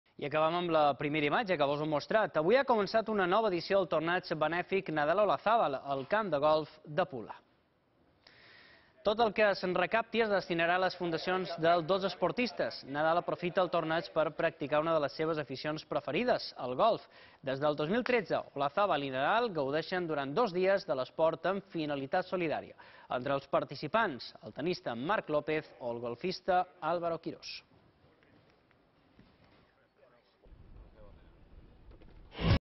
Comença el torneig benèfic Nadal-Olazábal a Pula
I avui ha començat una nova edició del torneig benèfic Nadal-Olazábal, al camp de golf de Pula. El que se recapti, es destinarà a les fundacions dels dos esportistes. Nadal aprofita el torneig per practicar una de les seves aficions preferides, el golf. Des del 2013 Olazábal i Nadal gaudeixen durant 2 dies de l’esport amb finalitat solidària. Entre els participants, el tennista Marc López o el golfista Alvaro Quirós.